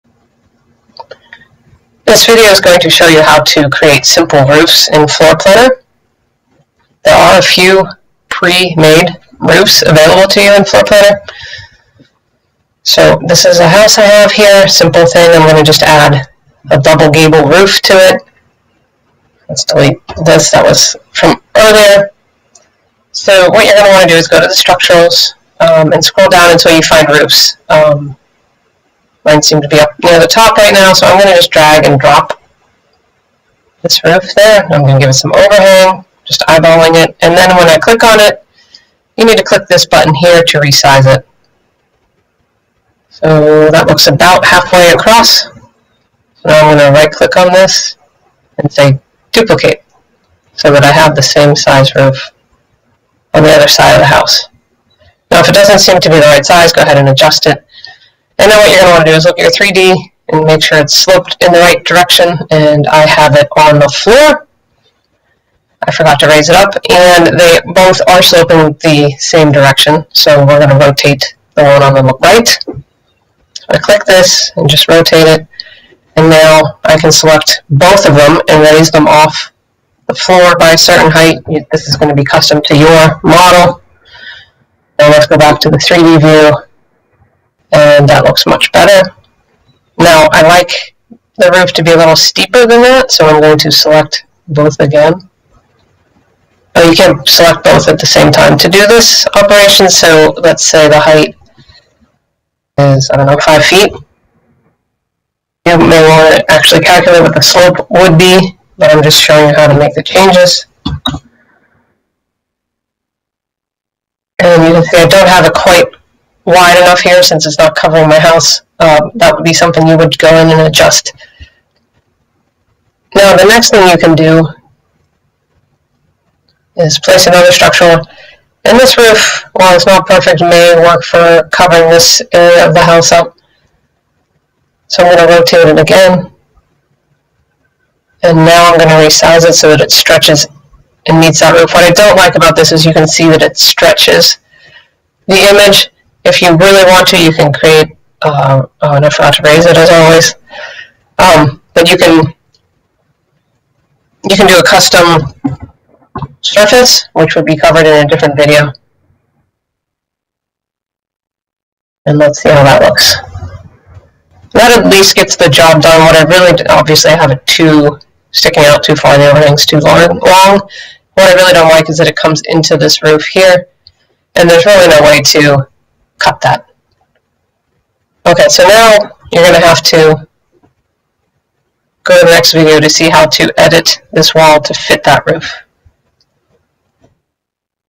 Simple Roofs in Floorplanner
add gable roof to floorplanner house